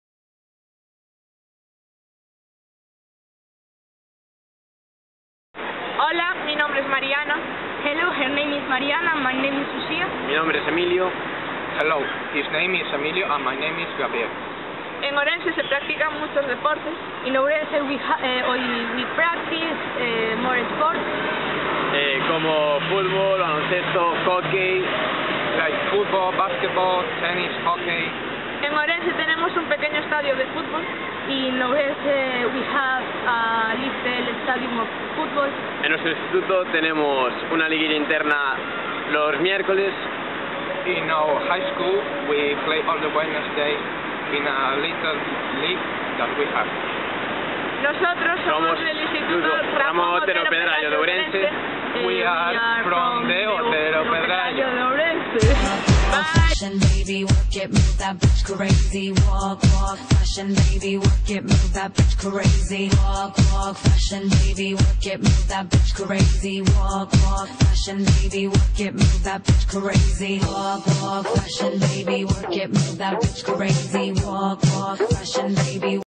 1ºE does Sports
Some students tell us about Ourense and sports.